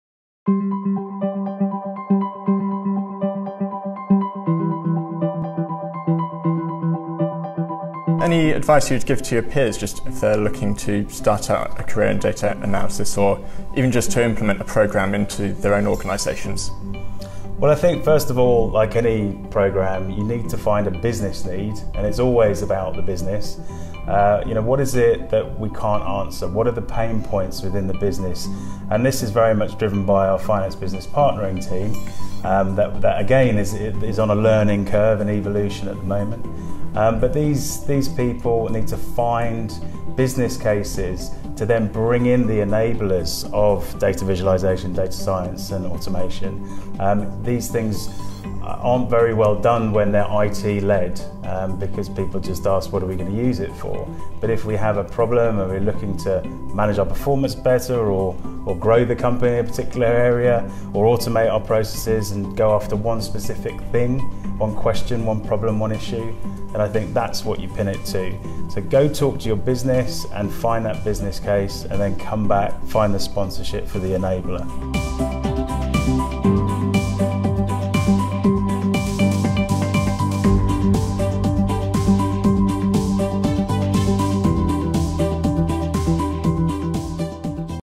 What advice do you have for those starting their data analysis careers?
Join Daniel Evans talking to Christopher Argent at the Finance Transformation conference in London (November 2018).